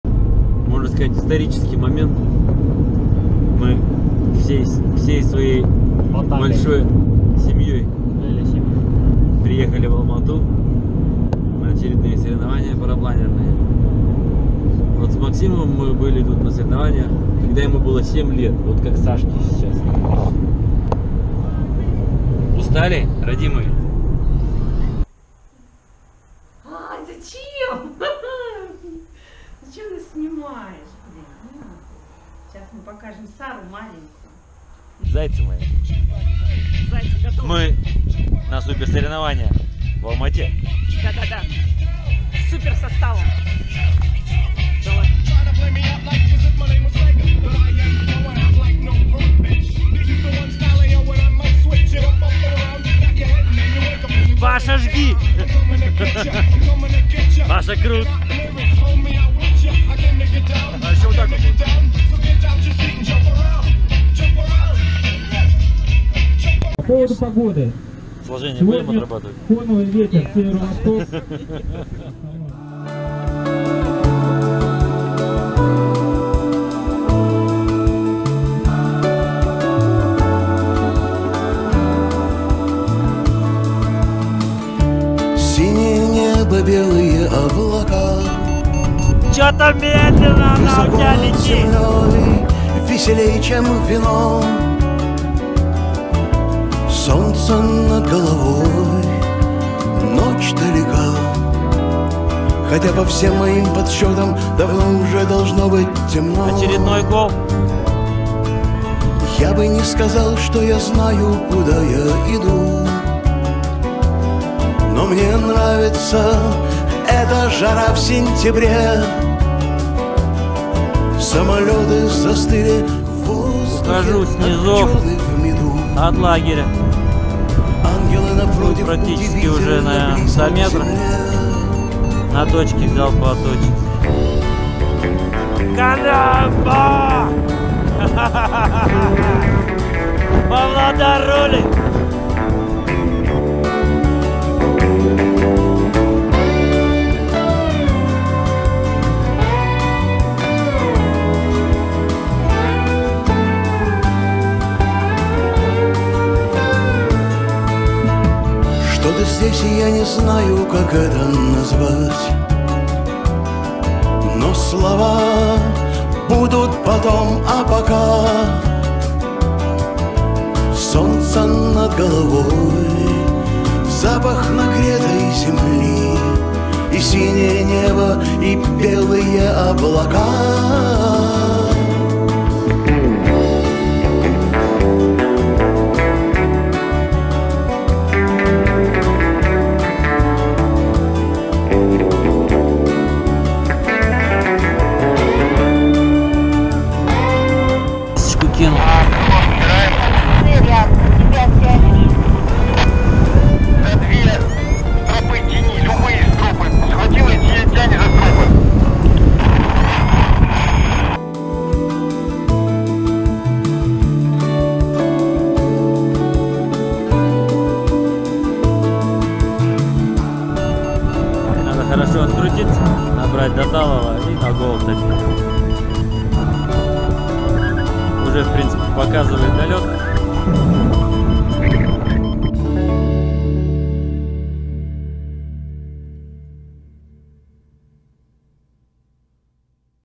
paragliding 2017 Open Kazakhstan Championship
Соревнования  по кросс кантри  полетам на парапланах .  Алматы   , Ушунур  .